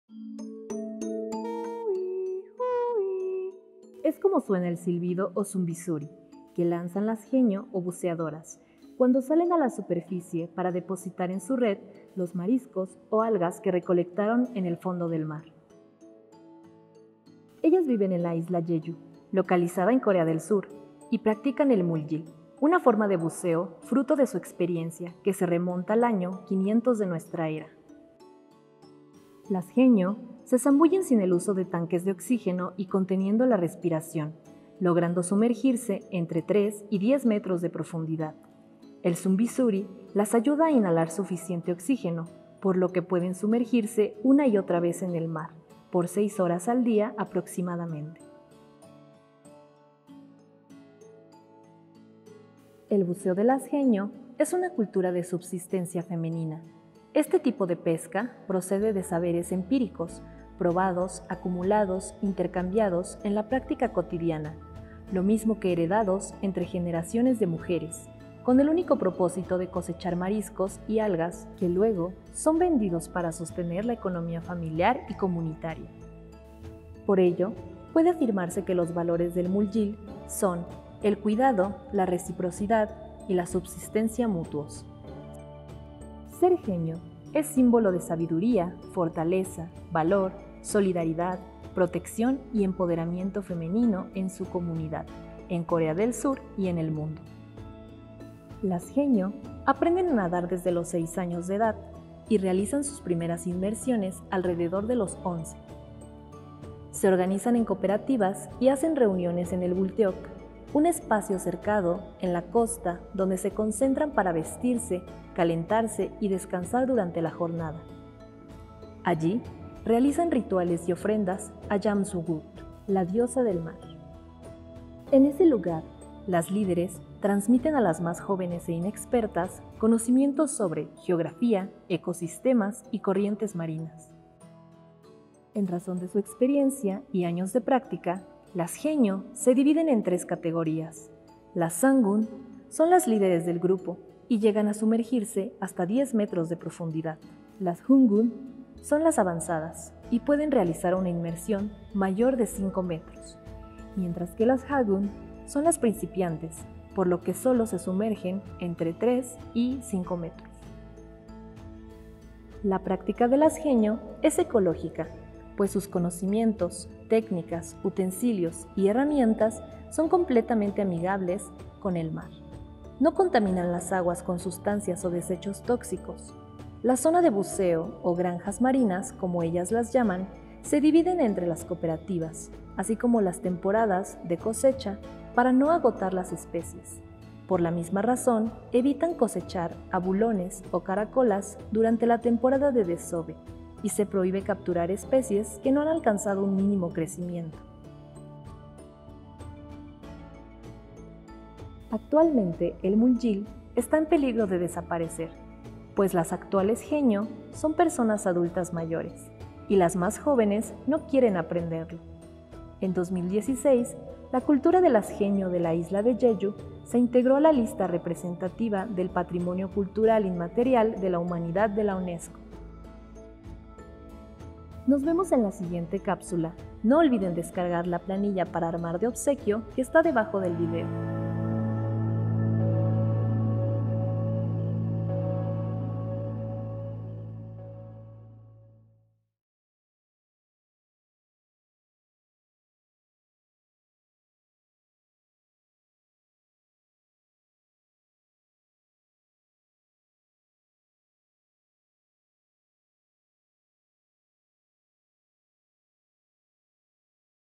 El mundo en una capsula HAENYEO
En la isla Jeju, en Corea del Sur, existe un grupo de mujeres que practica el buceo libre. Son conocidas como "Haenyeo" o "buceadoras", quienes obtienen su subsistencia del mar. Acompáñanos en este recorrido por su peculiar forma de vida y tradiciones.

En el marco del